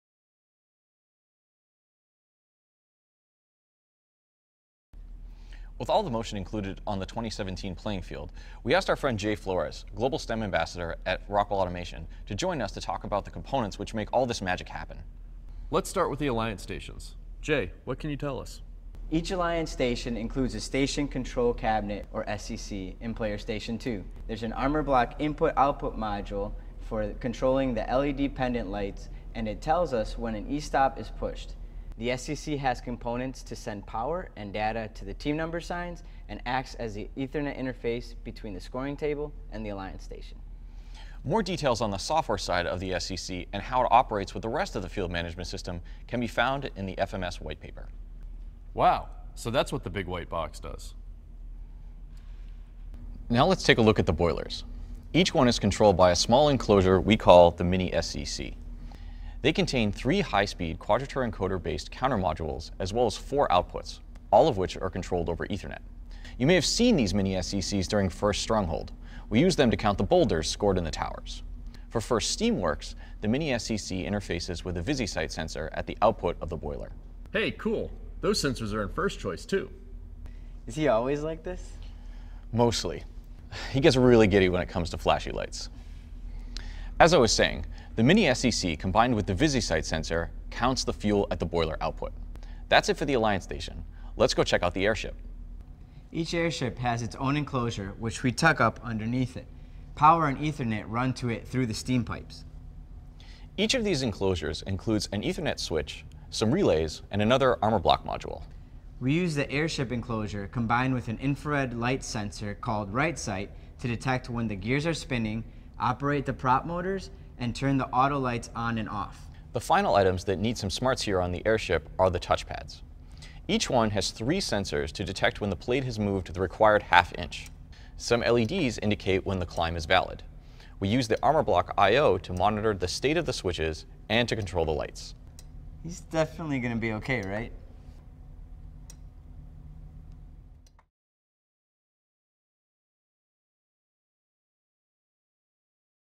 2017 Field Tour Video: Station Controls
FIRST Robotics Competition STEAMWORKS Station Controls Field Tour Video. Matt Pilotte, Derek Foster, and Jay Flores.